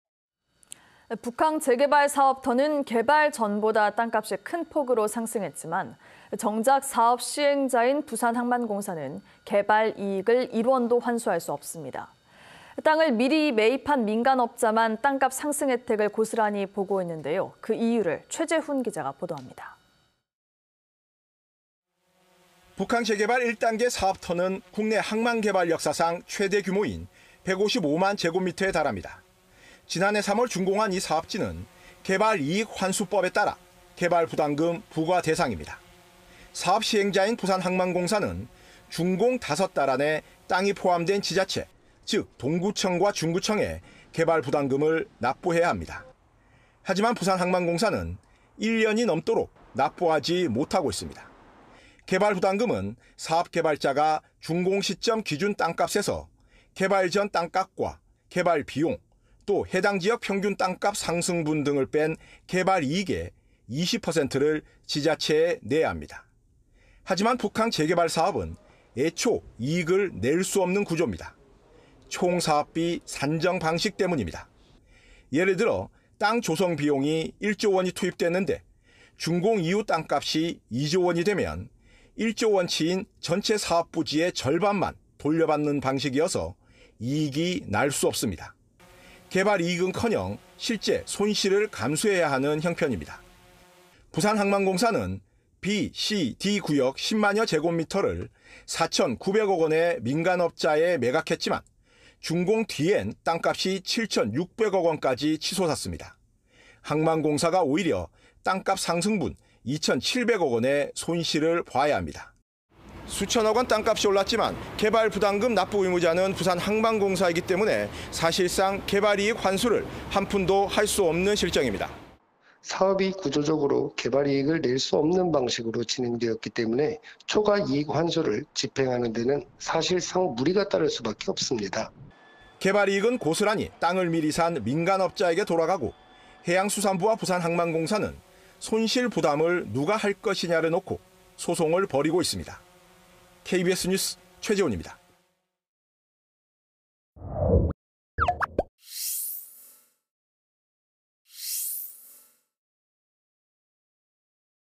북항재개발 이익 환수 ‘0원’…이유는? / KBS 2024.07.19.
[앵커]

 북항재개발 사업 터는 개발 전보다 땅값이 큰 폭으로 상승했지만 정작 사업 시행자인 부산항만공사는 개발 이익을 1원도 환수할 수 없습니다.

 땅을 미리 매입한 민간업자만 땅값 상승 혜택을 고스란히 보고 있는데, 그 이유를  최재훈 기자가 보도합니다.

 [리포트]

 북항재개발 1단계 사업 터는 국내 항만개발 역사상 최대 규모인 155만 제곱미터에 달합니다.

 지난해 3월 준공한 이 사업지는  '개발이익환수법'에 따라  개발부담금 부과 대상입니다.

 사업 시행자인 부산항만공사는 준공 다섯 달 안에 땅이 포함된 지자체,  즉 동구청과 중구청에  개발부담금을 납부해야 합니다.

 하지만 부산항만공사는 1년이 다 되도록 납부하지 못하고 있습니다.

 개발부담금은  사업 개발자가 준공 시점 기준 땅값에서 개발 전 땅값과 개발 비용, 또 해당 지역 평균 땅값 상승분 등을 뺀 개발 이익의 20%를 지자체에 내야 합니다.

 하지만 북항재개발 사업은 애초 이익을 낼 수 없는 구조입니다.

 총사업비 산정방식 때문입니다.

 예를 들어 땅 조성 비용이 1조 원이 투입됐는데, 준공 이후 땅값이 2조 원이 되면 1조 원치인 전체사업부지의 절반만  돌려받는 방식이어서 이익이 날 수 없습니다.

 개발이익은 커녕 실제 손실을 감수해야 하는 형편입니다.

 부산항만공사는  B,C,D 구역 10만여 제곱미터를 4,900억 원에 민간업자에 매각했지만 준공 뒤엔 땅값이 7,600억까지 치솟았습니다.

 항만공사가 오히려 땅값 상승분 2,700억 원의 손실을 봐야 합니다.

수천억 원 땅값이 올랐지만  개발부담금 납부의무자는 부산항만공사이기 때문에  사실상 개발이익 환수를 한 푼도 할 수 없는 실정입니다.

[조용복/동아대 경영대학 교수 : "사업이 구조적으로 개발 이익을 낼 수 없는 방식으로 진행되었기 때문에, 초과이익 환수를 집행하는 데는 사실상 무리가 따를 수밖에 없습니다.“]

 개발 이익은 고스란히 땅을 미리 산 민간업자에게 돌아가고  해양수산부부와 부산항만공사는 손실 부담을 누가 할 것이냐를 놓고 소송을 벌이고 있습니다.

 KBS 뉴스 최재훈입니다.

 촬영기자:류석민/영상편집:김종수/그래픽:김희나

[북항재개발사업 관련 뉴스 영상 다시 보기]